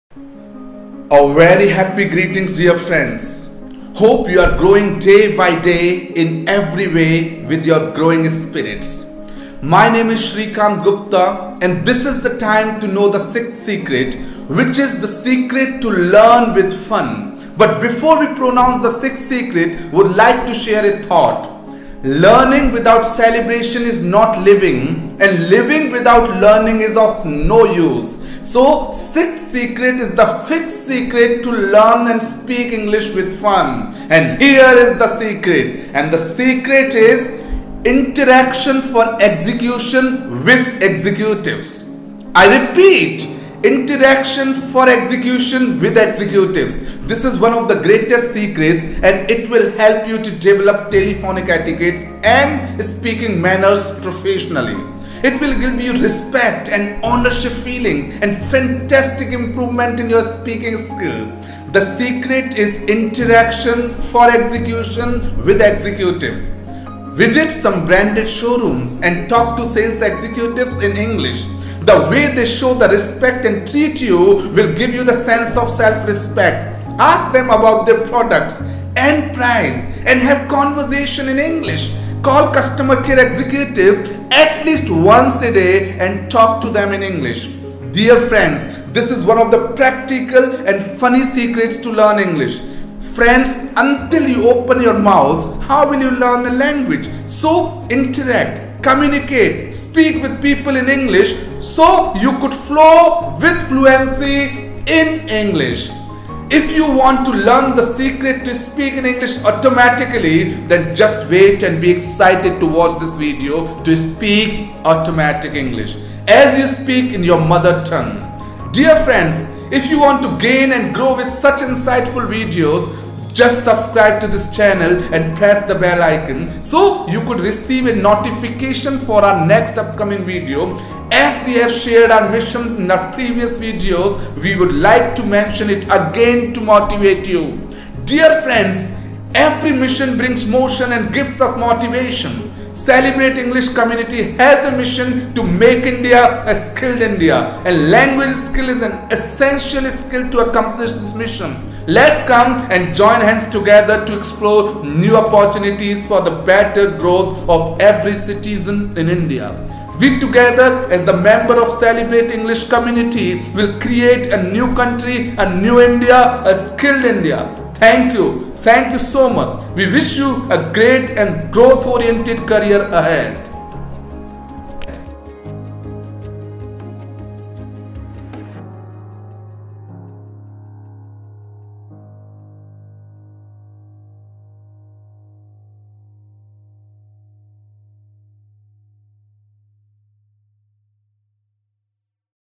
SECRET 6 | SPEAK ENGLISH | FLUENT ENGLISH
There is always a solution, there is always a way, and the way is on the way.

Congratulations on taking this step towards being a fluent English Speaker. Many students wonder if it is possible to speak English fluently, like a native speaker.
And the answer is "YES!".
But the question also arises that "Is it easy?"; and the answer is "NO", 'but nothing is impossible'.

You just need to follow these 7 Super Secrets to speak English fluently.
We believe that you are watching these 7 Super Secrets because you are ready and willing to learn.